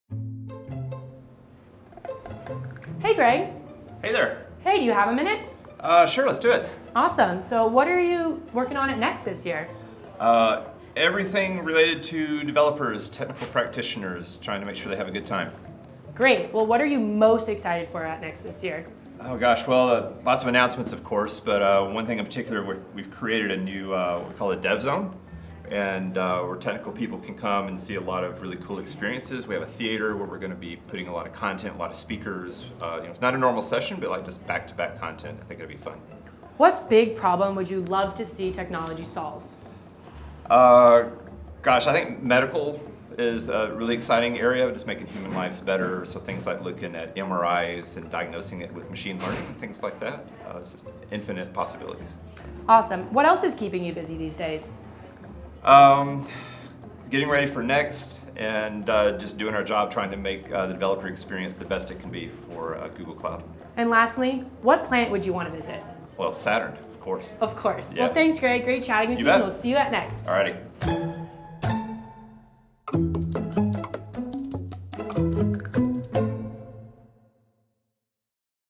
What to expect at Next 2019 - 60 seconds with Greg Wilson
In this episode of 60 seconds with a Googler, we sit down with Cloud Developer Relations Director, Greg Wilson. Watch to find out about Dev Zone at Google Cloud Next ‘19 and how his team helps make the developer experience awesome for everyone involved.